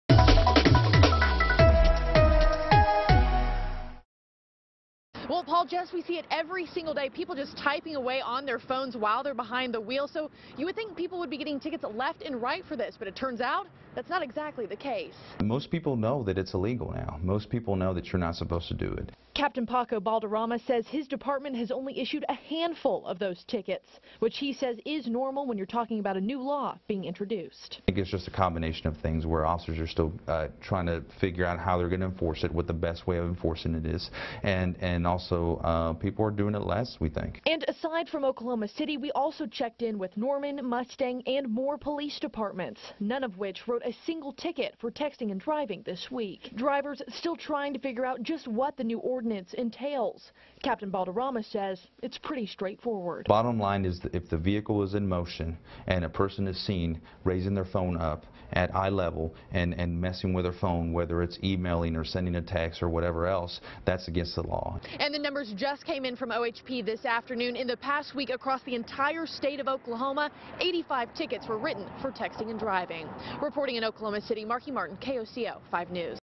Are police busy writing tickets after new texting and driving ban?
Oklahoma City police said they have not issued many citations for texting and driving since the law went into effect Nov. 1. Authorities credit the low citation numbers to the fact that citizens know that texting and driving is illegal.